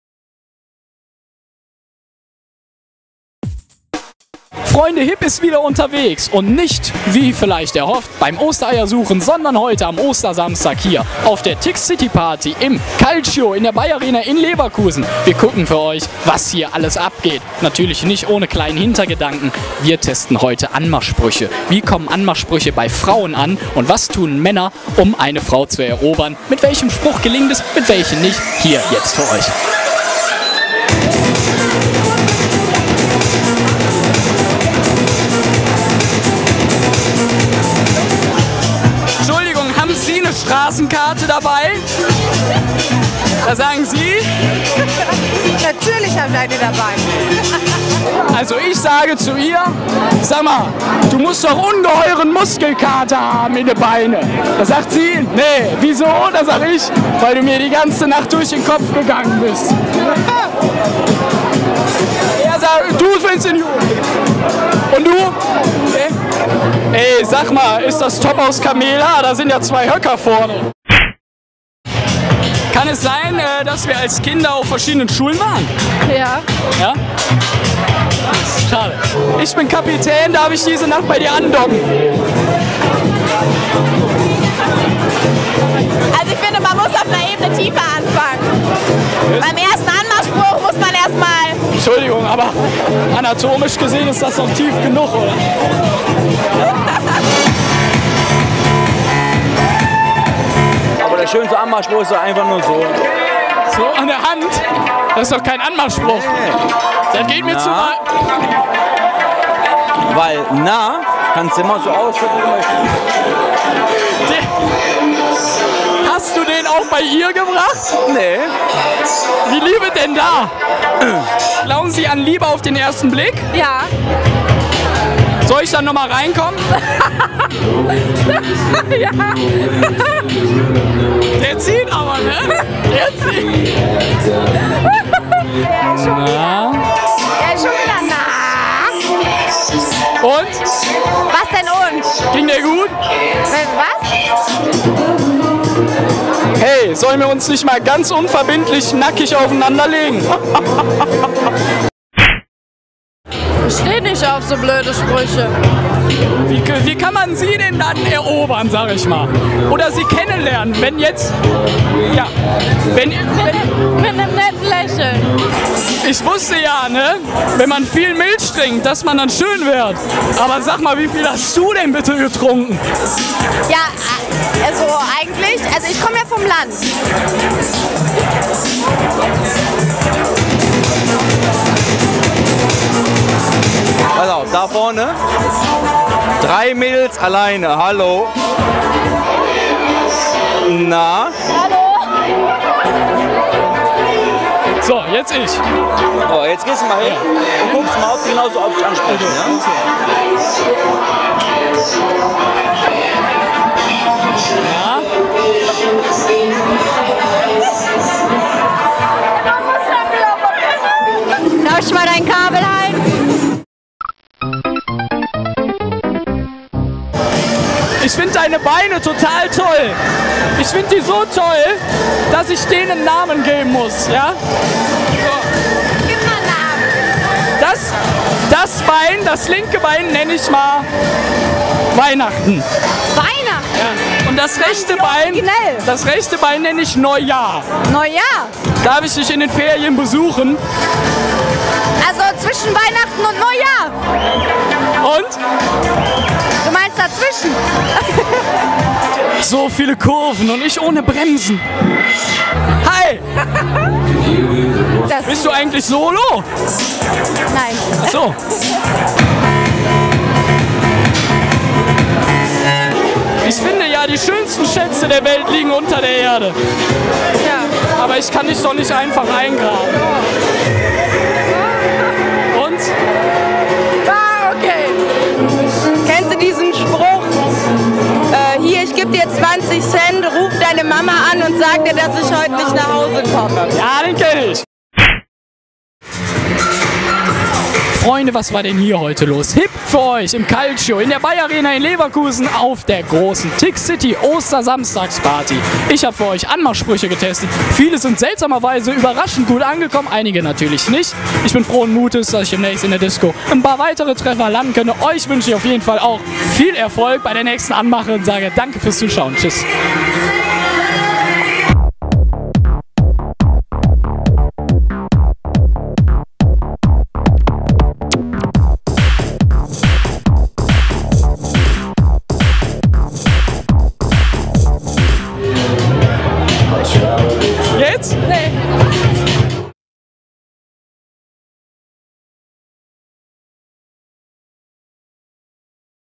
HiB auf Anmach-Party - Ey, haste gleich schon was vor?
An Ostern ist der Flirtfaktor bestimmt besonders hoch. Dachten wir zumindest. Deswegen ab zur ticCity-Party im Leverkusener Calcio und rein ins Haifischbecken der Anmachen.

Doch kommen Anmachsprüche wie 'Hast Du mal einen Stift, damit ich mir Deine Nummer aufschreiben kann?' wirklich bei der Frauenwelt an und welche Anmach-Asse schütteln Männer aus dem Ärmel, um bei einer Frau zu landen?

Wir testeten für Euch einige Hochkaräter - mit überraschendem Ergebnis...